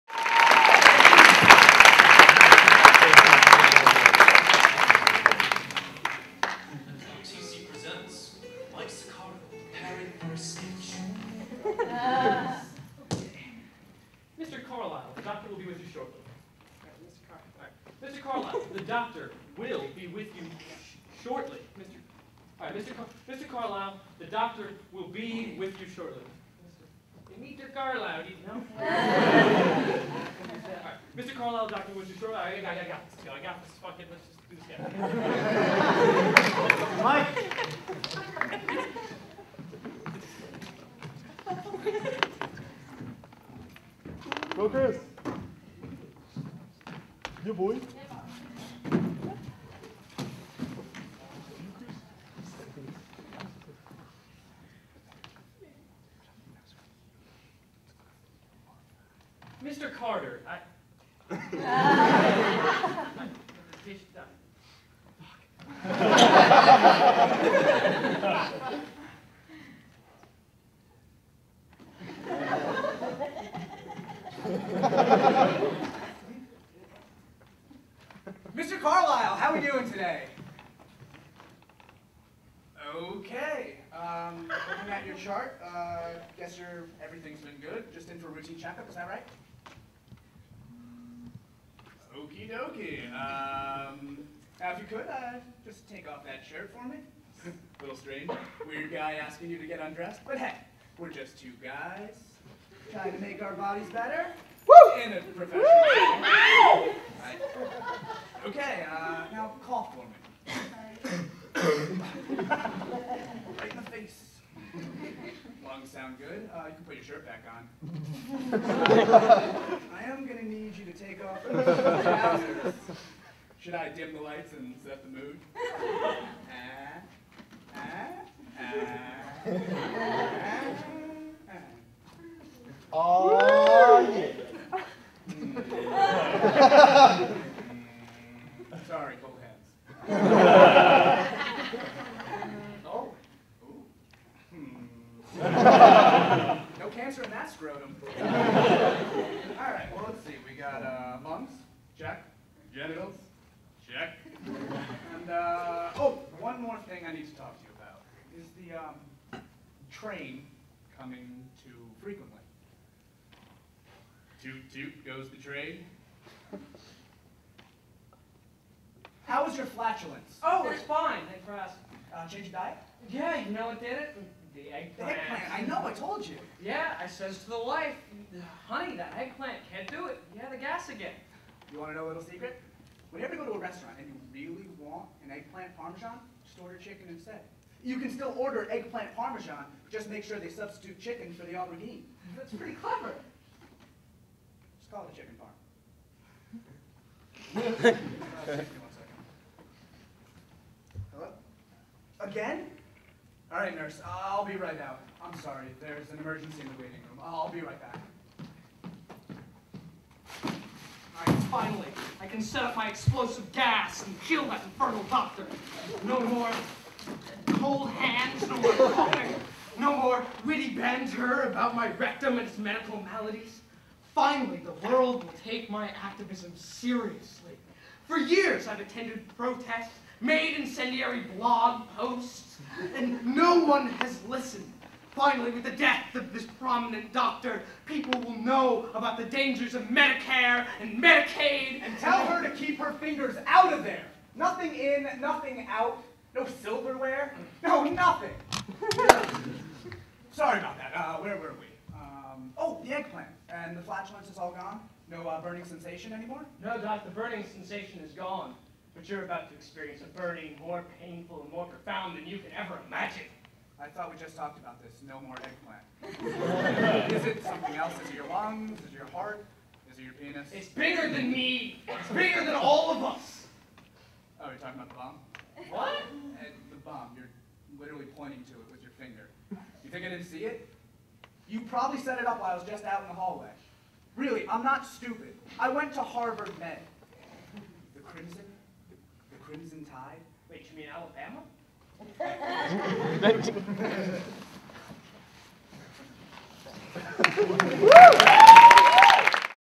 TC 2009 24 hour Show PART 2 of 8
Throat Culture - Johns Hopkins University's only sketch comedy group - presents the 2009 24 Hour Show!  The 24 hour show is a yearly show written, rehearsed, and performed within a 24 hour period, all based on audience prompts posted on Facebook.